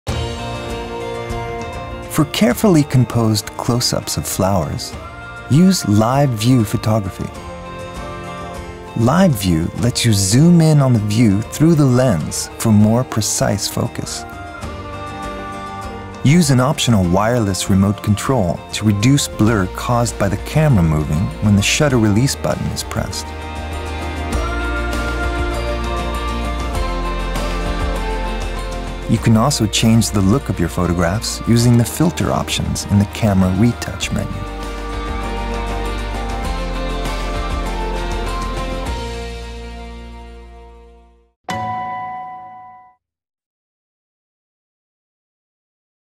Advanced photography: using live-view when shooting flowers close-up
Discover how to get more precise focus on your floral shots by using live view, and learn to change the look of your images by using the filter options in your DSLR's retouch menu.